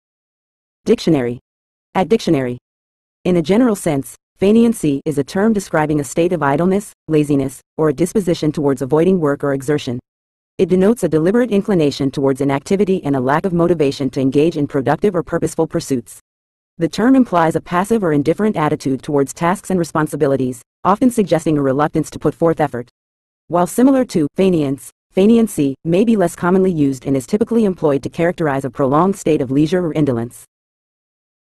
Faineancy Meaning In English
In a general sense, "faineancy" is a term describing a state of idleness, laziness, or a disposition towards avoiding work or exertion. It denotes a deliberate inclination towards inactivity and a lack of motivation to engage in productive or purposeful pursuits. The term implies a passive or indifferent attitude towards tasks and responsibilities, often suggesting a reluctance to put forth effort. While similar to "faineance," "faineancy" may be less commonly used and is typically employed to characterize a prolonged state of leisure or indolence.